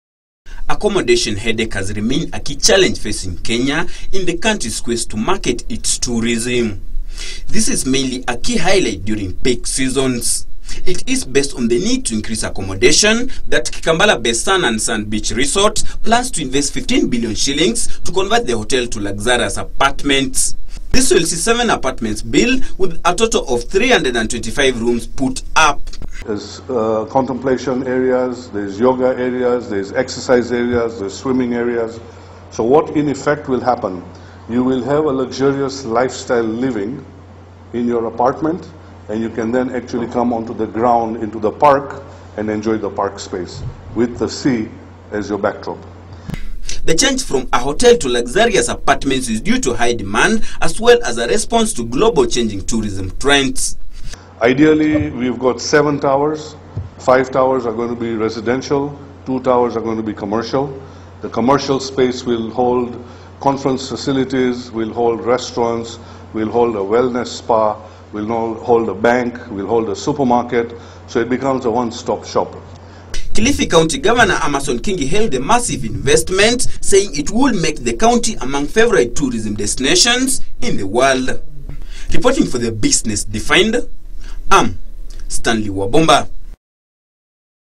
Sun & Sand Beach Resort to invest 15 Billion Shillings to convert the hotel to luxurious apartments
The management of Kikambala based Sun and Sand Beach Resort plan to invest 15 billion shillings to convert the hotel to luxurious apartments. The Resort's Director Mahmud Visram says they plan to construct seven apartments with a total of 325 rooms under the name Ocean Seven which will then be sold to investors. Reported by Stanley Wabomba.